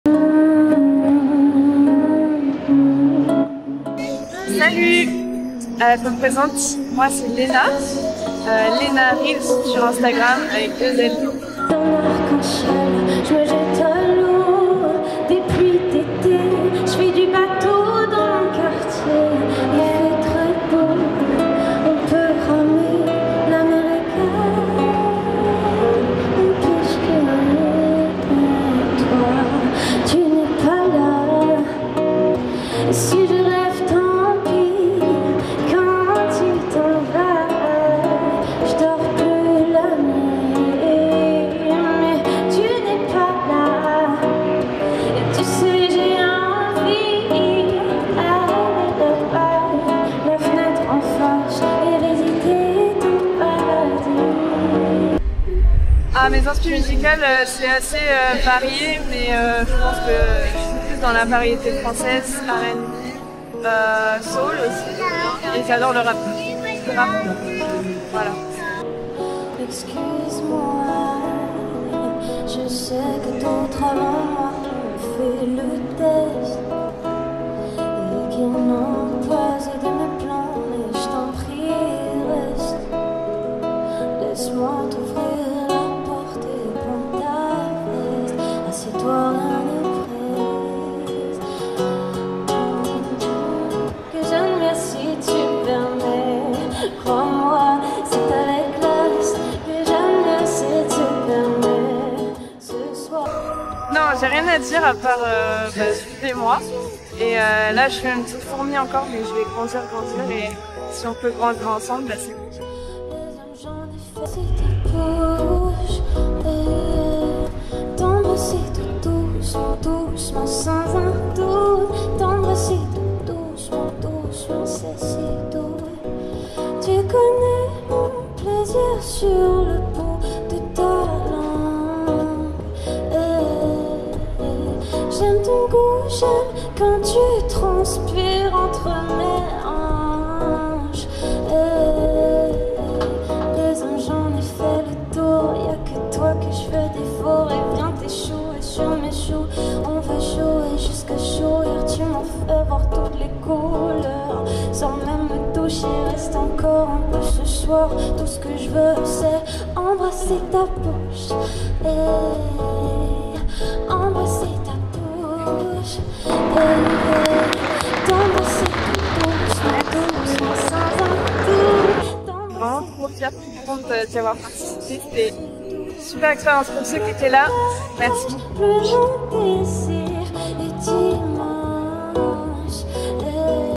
Talents des Gares - Saint Lazare - Lena [LPE]
Le concours « Talents des Gares » est un tremplin à destination des artistes et des groupes de musique organisé par LPE et SNCF Gares & Connexions en gare Saint-Lazare.

Le tremplin s’organise en 3 étapes : candidatures / qualifications, demi-finales / prestations scéniques en gare, vote final délivré par un jury de professionnels de la musique et d’agents SNCF.

L’objectif de ce concours est de permettre aux artistes et groupes de pouvoir exprimer leurs talents en direct et d’être récompensés par des lots en lien avec la musique.

Vous chantez et vous êtes passionnés de musique ?
Dévoilez votre talent en gare Saint-Lazare !